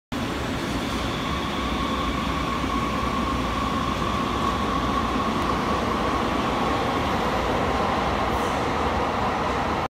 2021 Fastest train in the world l god gifted beauty l world's largest high-speed railway network
Learn some of the high-tech engineering behind the Shinkansen, and how the trains are kept so precisely on schedule, including the super speedy turn around at Tokyo Station.
China already possesses the world's largest high-speed railway network, and it will only expand more ambitiously, according to the country's new Five-Year Plan. But it's not just the railroads that are extending. China may also get a major boost in the speed of its bullet trains. A prototype maglev train unveiled recently at Southwest Jiao tong University in Chengdu aims to set a new speed record for trains, the current record of 608 kilometers per hour is held by Japan's SC Maglev. China's answer to it is 628.

# godgiftedbeauty